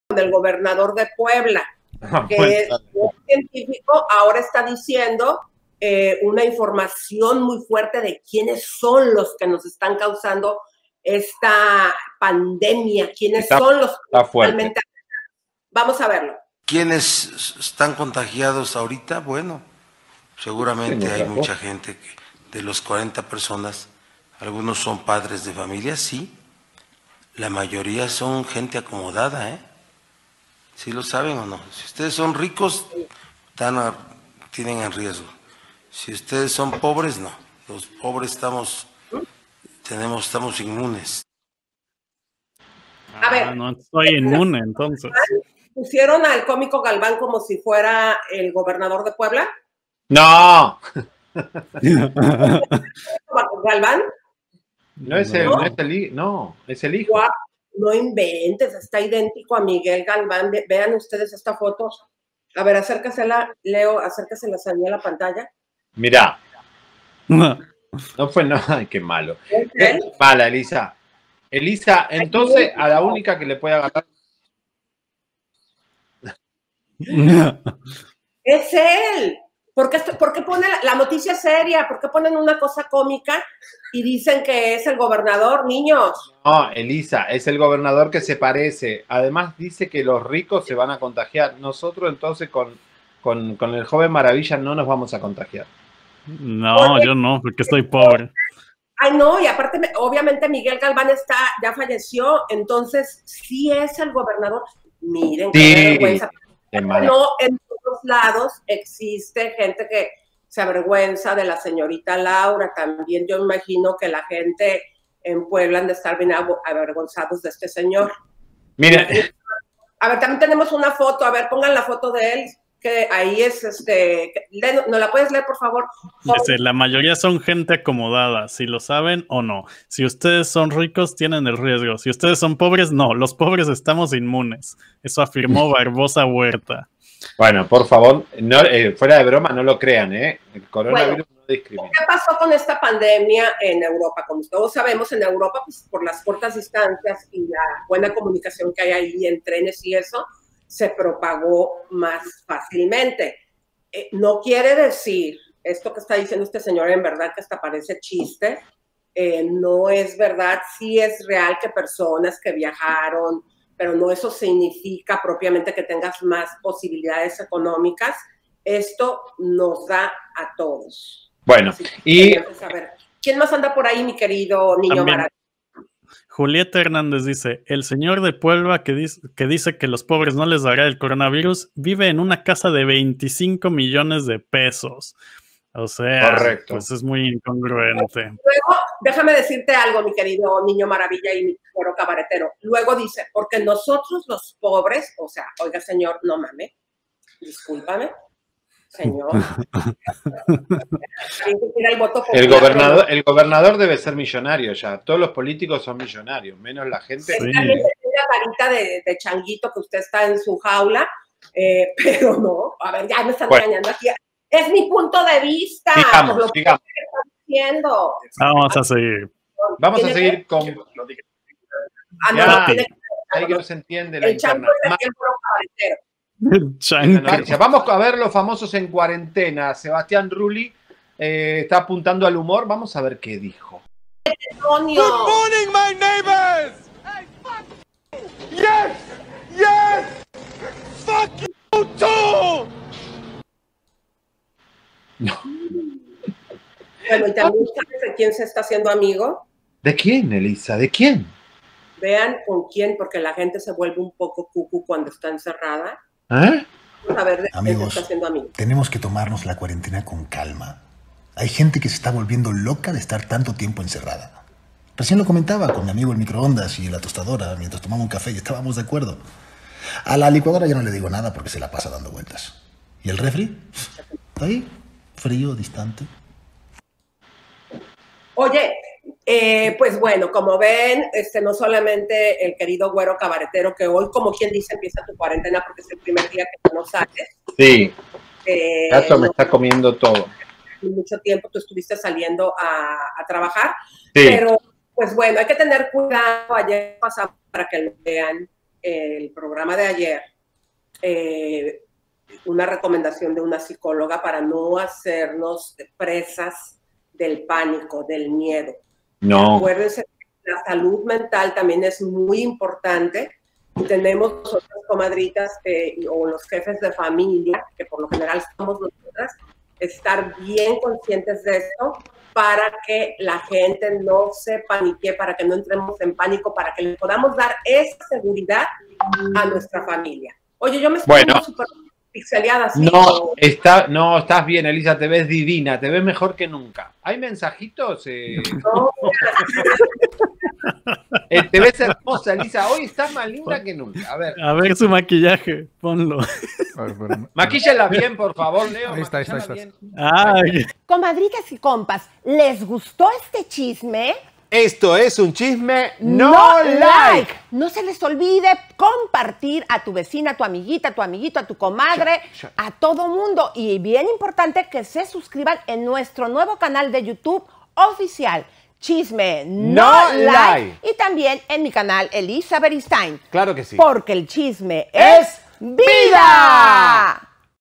POBRES SON INMUNES A LA ENFERMEDAD SEGÚN GOBERNADOR DE PUEBLA - CHISME NO LIKE
El Gobernador de Puebla vuelve a poner a México como el hazme reír del mundo ante esta enfermedad. Ahora dice que los pobres no se pueden enfermar porque son inmunes y que los ricos son los más afectados por viajar tanto.